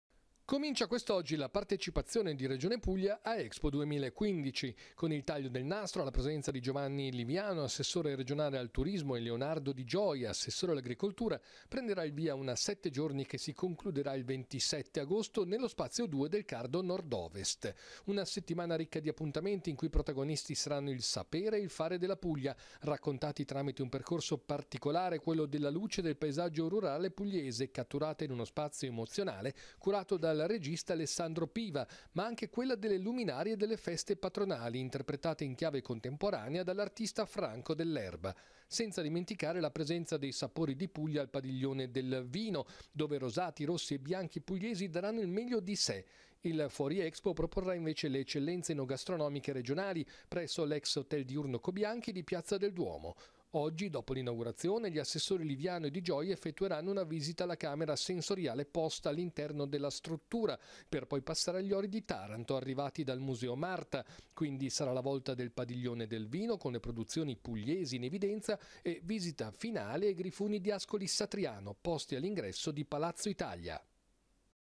MILANO APERTURA PADIGLIONE DELLA REGIONE PUGLIA
LA REGIONE PUGLIA ALL'EXPO DI MILANO. OGGI POMERIGGIO L'APERTURA DEL PADIGLIONE, ALLA PRESENZA DEGLI ASSESSORI AL TURISMO LIVIANO E ALL'AGRICOLTURA DI GIOIA. SARA' UNA SETTIMANA RICCA DI APPUNTAMENTI, IN CUI I PROTAGONISTI SARANNO LA LUCE DEL PAESAGGIO PUGLIESE, CATTURATA IN UN PERCORSO EMOZIONALE CURATO DAL REGISTA ALESSANDRO PIVA, MA ANCHE LE LUMINARIE DELLE FESTE PATRONALI E NATURALMENTE I VINI ROSSI, BIANCHI E ROSE'. COSTO UN MILIONE DI EURO. ALL'EXPO SONO GIA' VISITABILI I SIMBOLI DELL'ARTE ANTICA PUGLIESE: UNA SELEZIONE DEGLI ORI DI TARANTO, PROVENIENTE DAL MUSEO MARTA', E I GRIFONI DI ASCOLI SATRIANO, CHE SI TROVANO ALL'INGRESSO DI PALAZZO ITALIA.